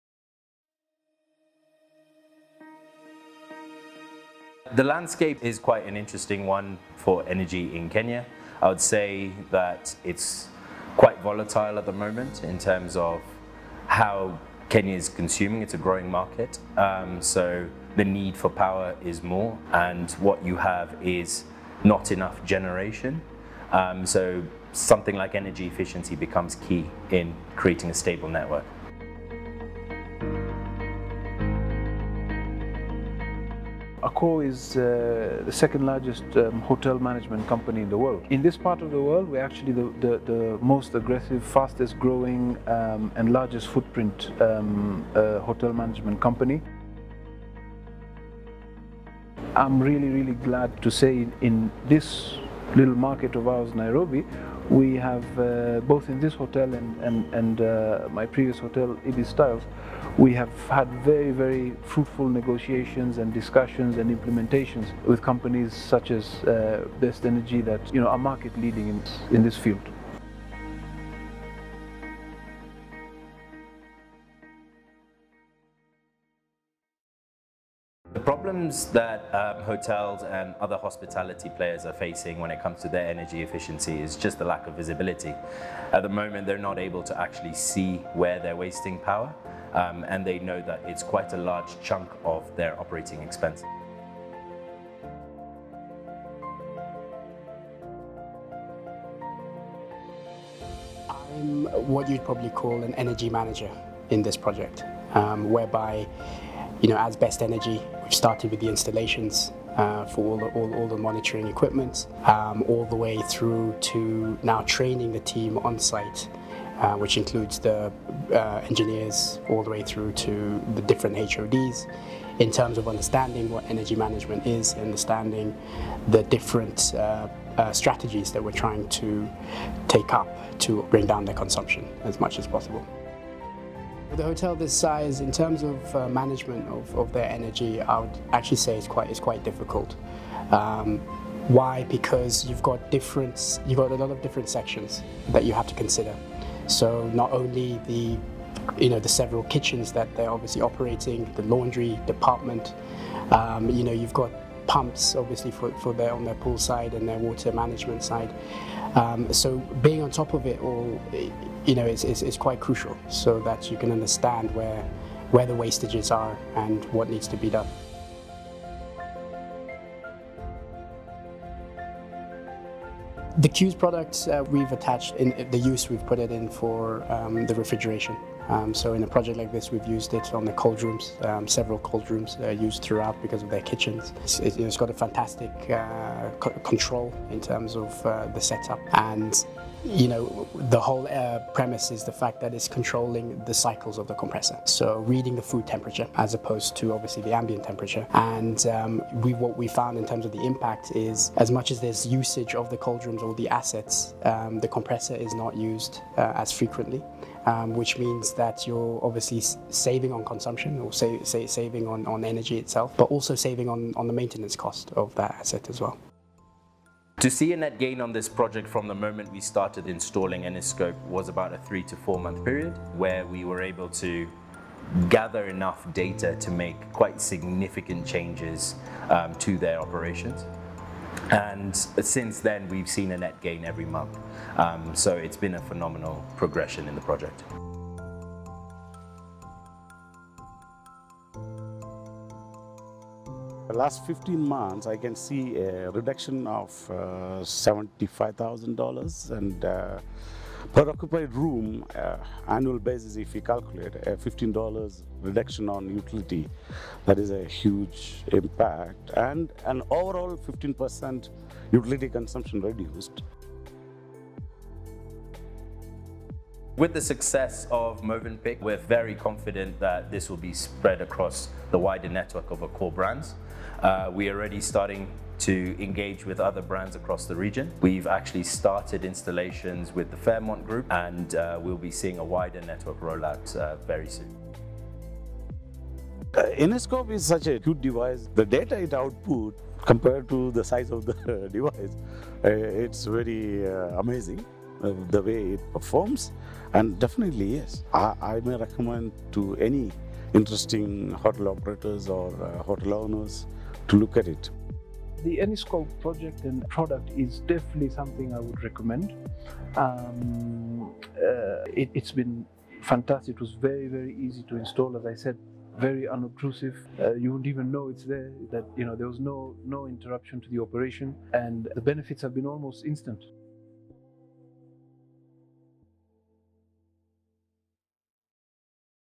Movenpick Hotel, Case Study - Eniscope Energy Management
Controlling energy waste in a hotel is a huge challenge. The complexity of the operation makes finding that waste, and eradicating it, hard to do - but rising energy costs make it more necessary than ever. So how can you use the Eniscope IoT Energy Management system to quickly, seamlessly and profitably solve the problem? Take a look at this case study with the Movenpick Hotel (Accor Group) in Nairobi to find out how!

PS. Interested in finding out more about the Movenpick Hotel? They were fantastic hosts during the filming of this case study and have shown a passionate commitment to energy saving. Check out their website here: